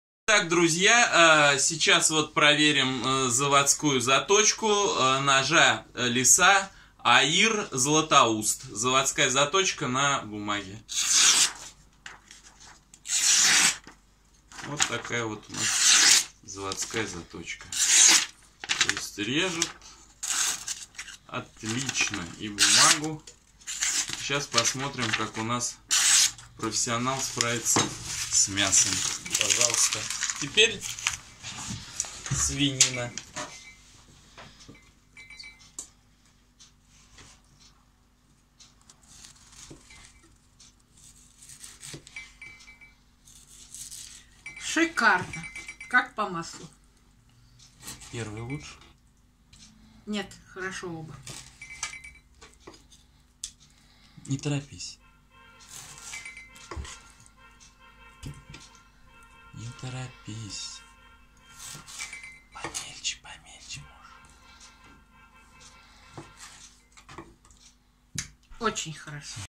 Нож Лиса 95х18 от Аир Златоуст рез
Нож Лиса 95х18 от Аир Златоуст рез по свинине.
мой адрес в контакте там Вы найдёте большой контент по ножам "Антон Уваров".